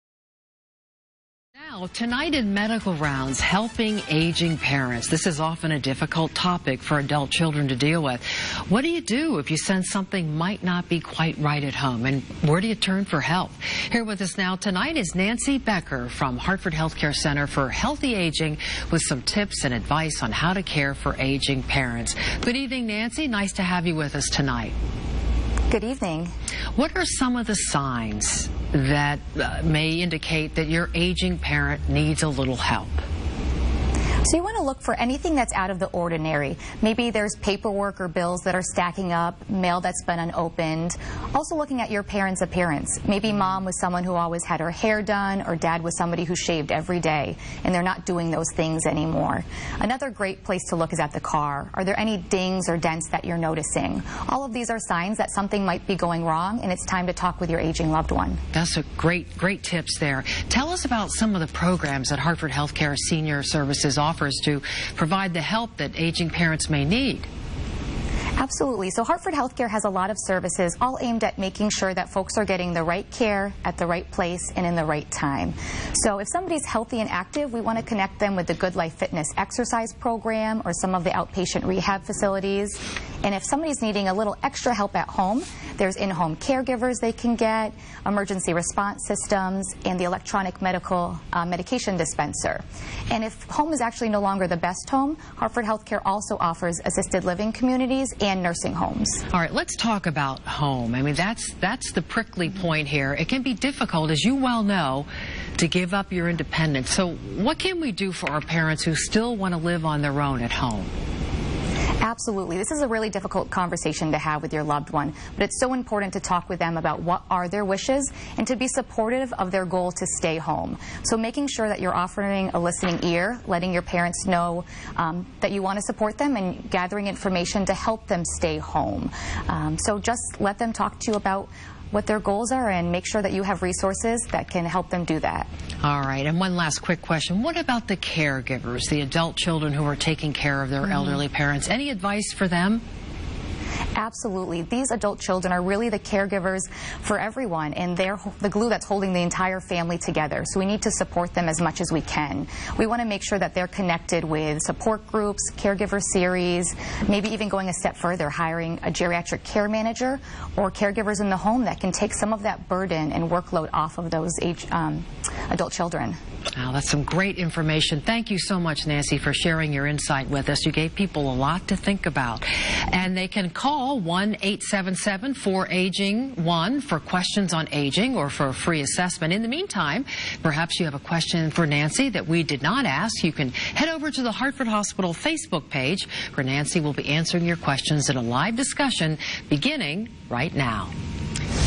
Medical Rounds with Nancy Becker
Helping aging parents can be stressful, particularly for their adult children. It's a difficult topic to bring up with older parents - especially if the adult children are sensing something is not quite right with their parents. Nancy Becker from Hartford HealthCare Center for Healthy Aging has some tips and advice on how to care for aging parents.